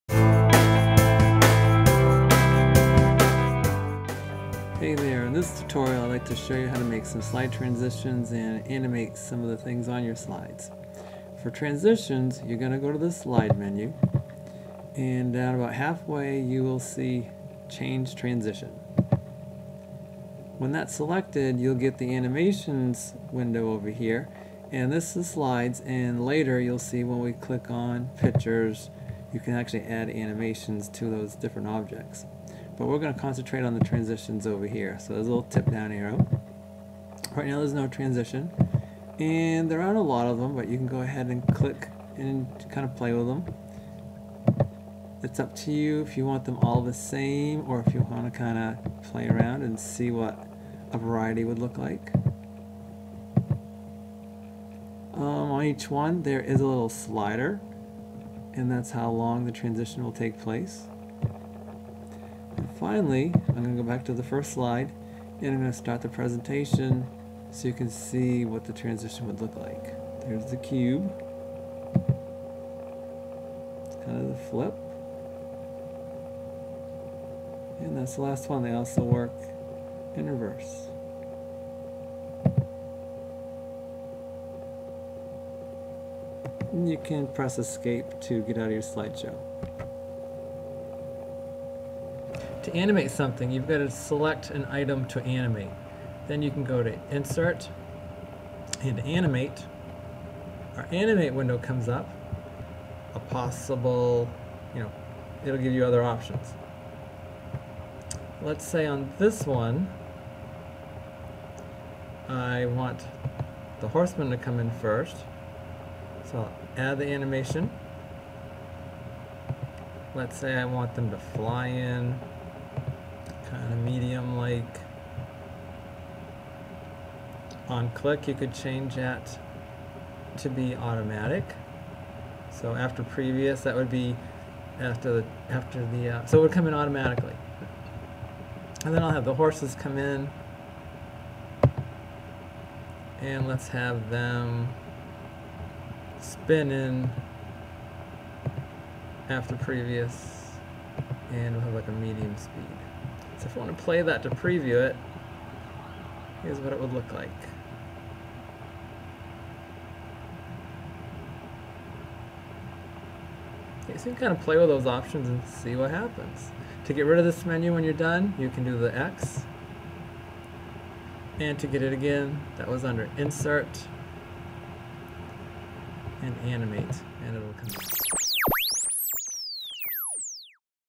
Google Presentation Transitions and Animations.m4v
This video shows how to add transitions and animations to your Google presentation.